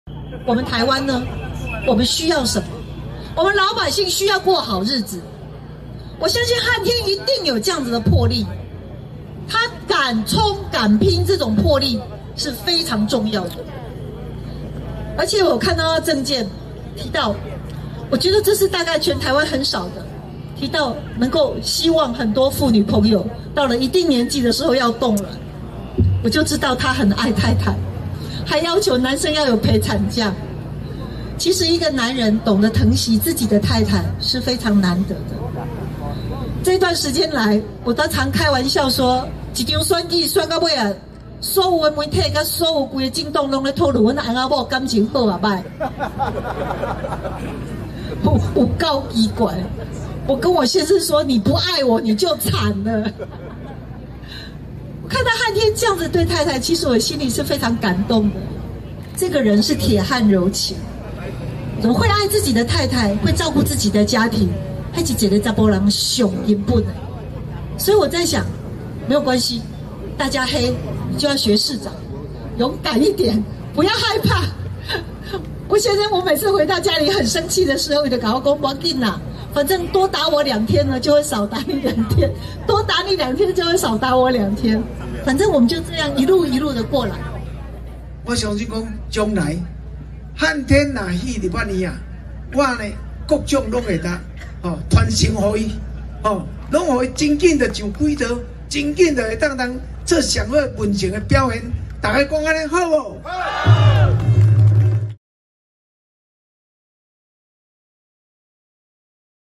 焦點都在他們之間？李佳芬喊話韓國瑜：你不愛我就慘了！
相關新聞：
就是不同框！李佳芬彰化站台前腳走 王金平才上台

國民黨總統候選人韓國瑜的妻子李佳芬，今天為彰化縣第一選區立委候選人張瀚天站台時強調，這次選舉選到後來，所有媒體與政黨都在討論他們夫妻感情，實在有夠奇怪，她都會跟韓國瑜說，你不愛我就慘了！